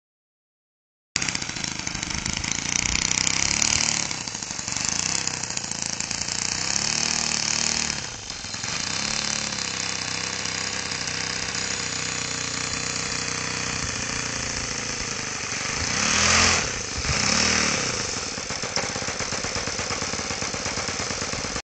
Taxi testing with new skis
Seagull Decathlon 120 taxi tests with YS 110 FZ engine and brand new skis.